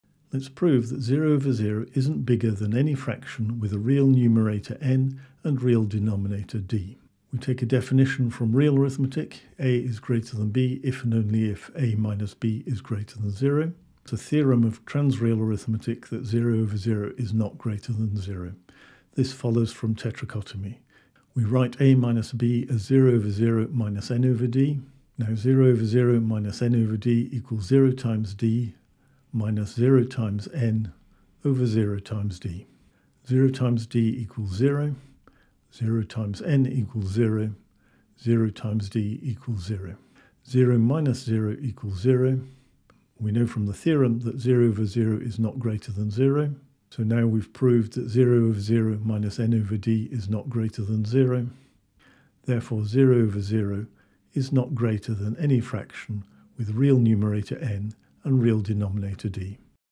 Why Isn’t 0/0 Big? No Calculus!
Algebraic proof that 0/0 is not greater than any fracterm with a real numerator and denominator. Does not use calculus.

LINKS